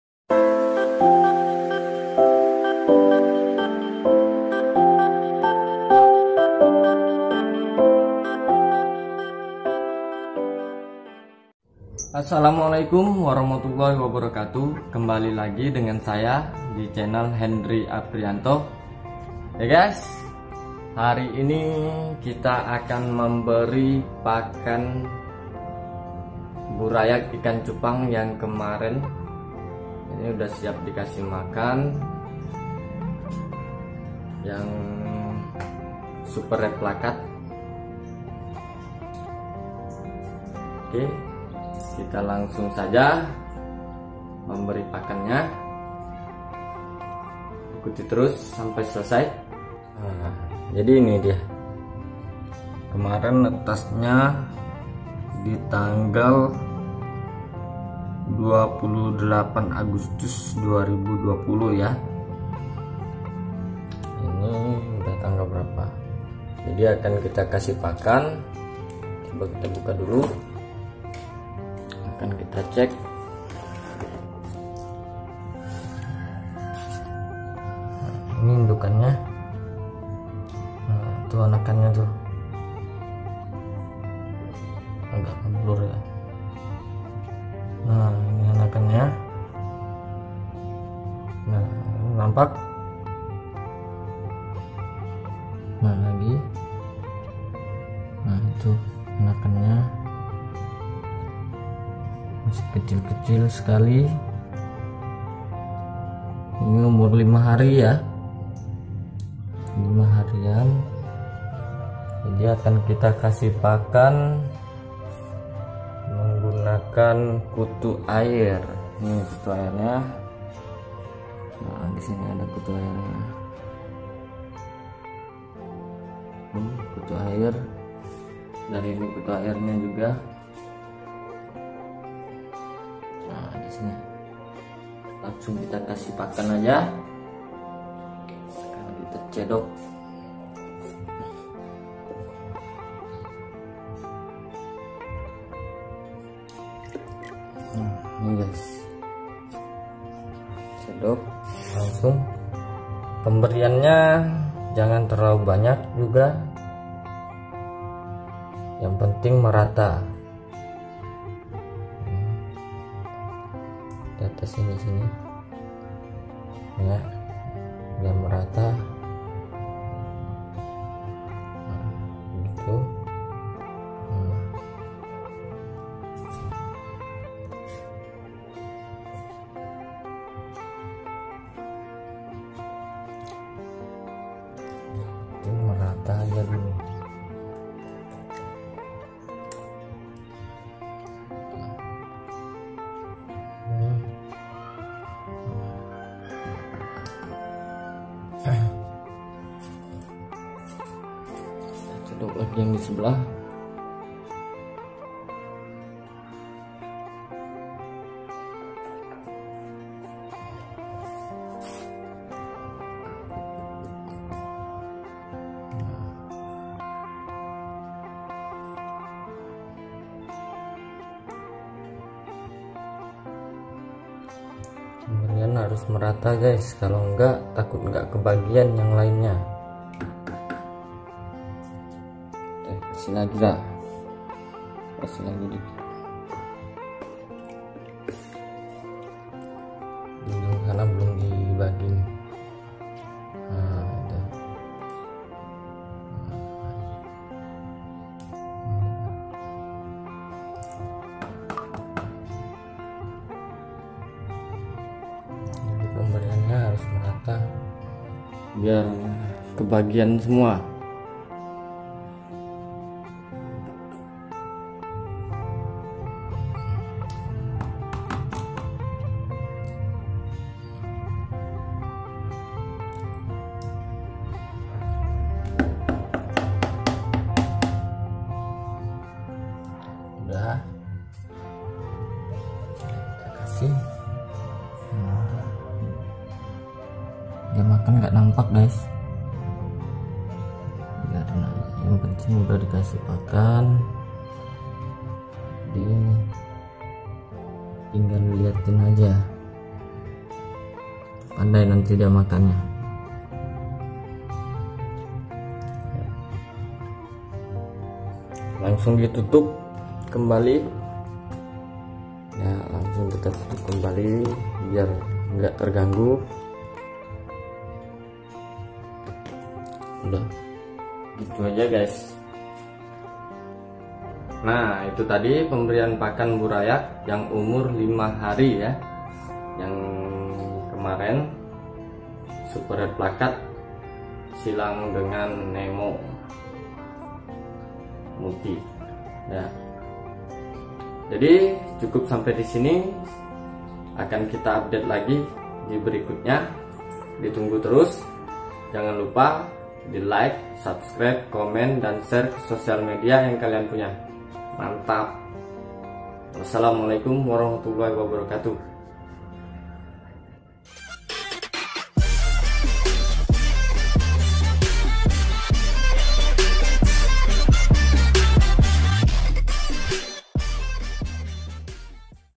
Tutorial Breeding Ikan Cupang Dari Awal Penjodohan Hingga Menghasilkan Burayak Part 4
Nah ini kelanjutan video kita yang kemaren di breeding yaa, kali ini kita akan memberi pakan pada burayak.. yok simak videonya sampai selesai
Dukung chanel saya dengan cara :
KOMENTAR
Dan jangan lupa aktifkan tombol lonceng notifikasinya agar selalu update di chanel Hendri Apriyanto
Terimakasih dan Semoga Bermanfaat